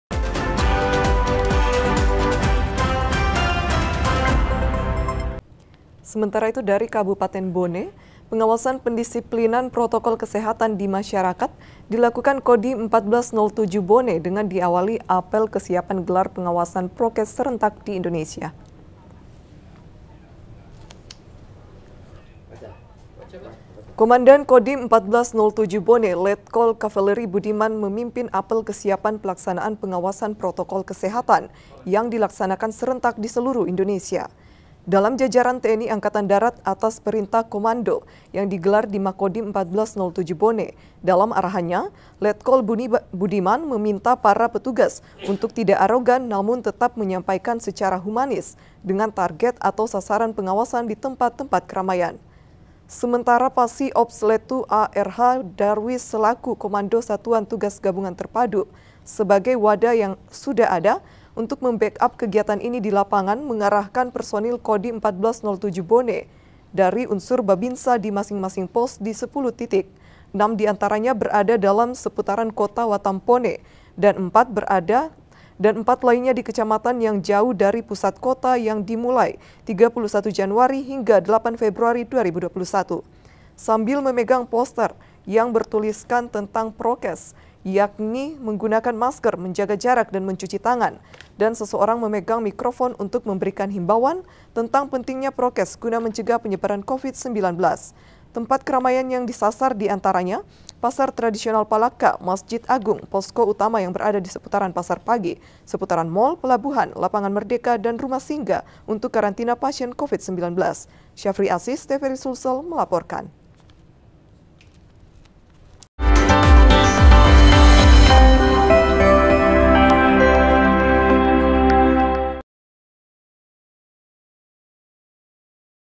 TNI AD Perketat Protokol Kesehatan di Tempat Keramaian
TNI AD Perketat Protokol Kesehatan di Tempat Keramaian Kabupaten Bone Syafri Azis TVRI Sulsel Mewartakan